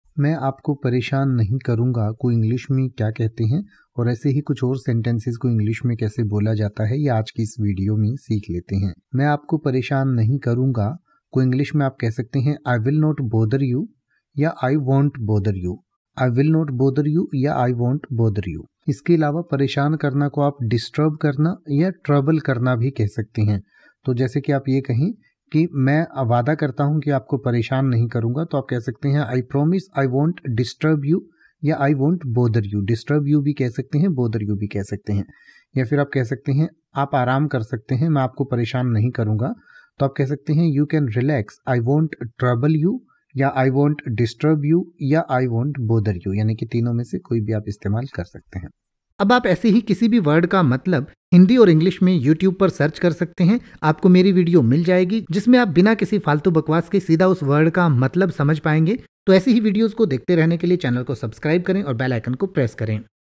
मैं आपको परेशान नहीं करूँगा को इंग्लिश में क्या कहते हैं| main aapko preshan nahi karunga ki English
इस वीडियो में आप मैं आपको परेशान नहीं करूँगा का हिंदी में मतलब समझेंगे और साथ में मैं आपको परेशान नहीं करूँगा का pronunciation भी सीखेंगें यानि की आप मैं आपको परेशान नहीं करूँगा के अर्थ के साथ साथ ये भी सीखेंगें की मैं आपको परेशान नहीं करूँगा को कैसे बोले या मैं आपको परेशान नहीं करूँगा को बोलने का सही तरीका क्या है |

तो बस एक मिनट में सीखिए मैं आपको परेशान नहीं करूँगा को|
Let’s learn Hindi Meaning of main aapko preshan nahi karunga in detail.

मैं आपको परेशान नहीं करूँगा का अंग्रेजी में अर्थ होता है
I will not bother you, I won't bother you

I promise I won't disturb you.
मैं वादा करता हूँ कि मैं आपको परेशान नहीं करूँगा।

You can relax; I won't trouble you.
आप आराम कर सकते हो; मैं आपको परेशान नहीं करूँगा।